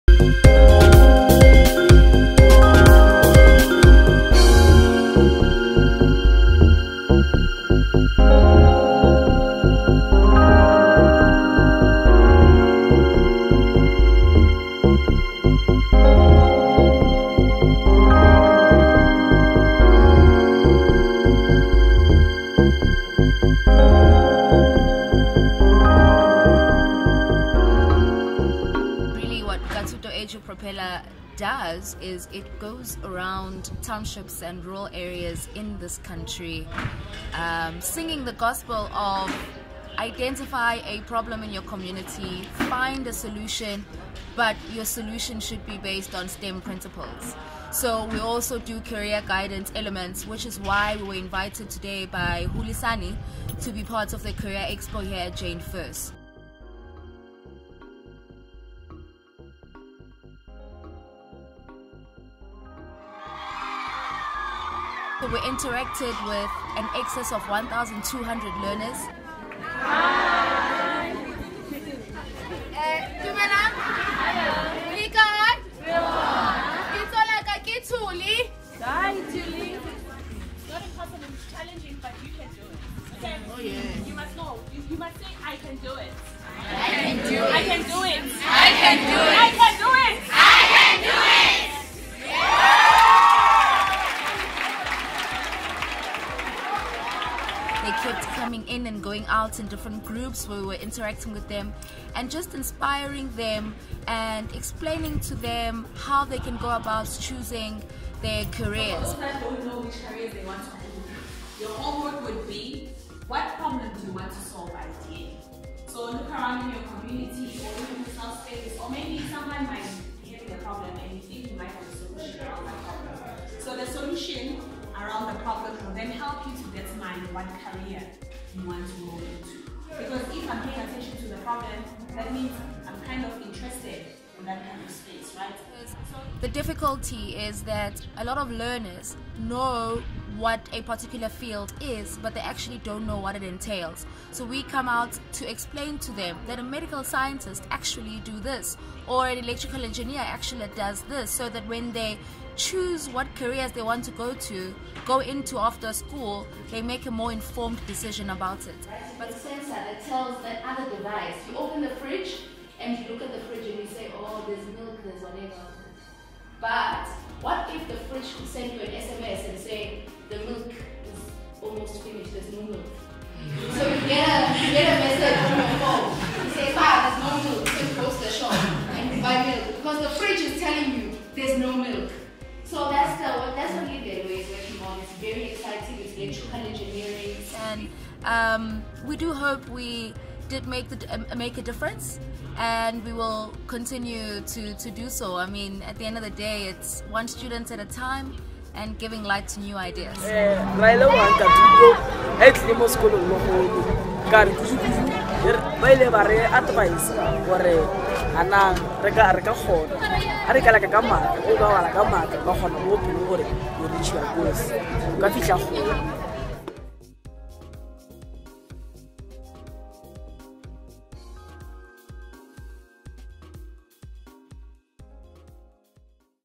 Nka'Thuto goes to Career Expo in Limpopo
Nka'Thuto took to Limpopo to interact with 1300 learners from the Hulisani Centre in Jane Furse. As per the mandate of Nka'Thuto, learners were encouraged to look into the problems in their communities to find areas of interest to them. It is ideal to pick the career you go into on how much you would enjoy doing that work on a daily basis. EduPropellers in Limpopo responded well and we wish them all the best in their studies until we see them again in the near future.